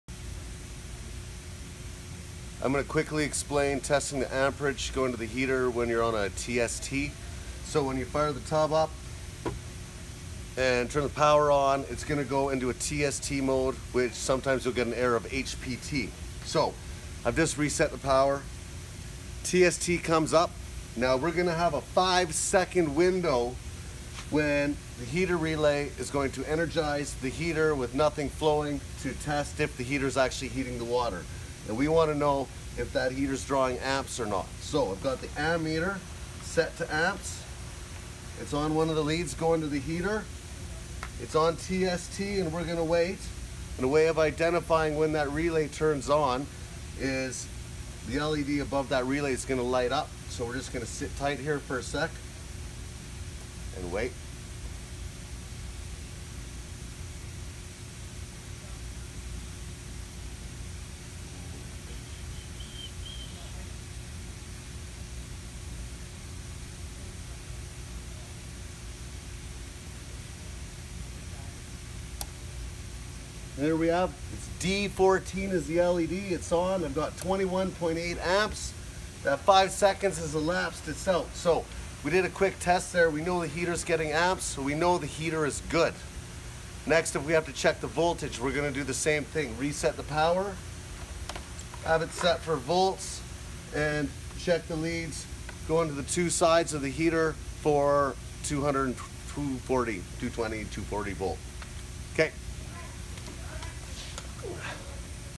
HPT error testing Amps to heater
during a TST test on start up the heater is energized for 5 seconds. during this 5 seconds we want to test for amp draw to the heater to confirm the heater good.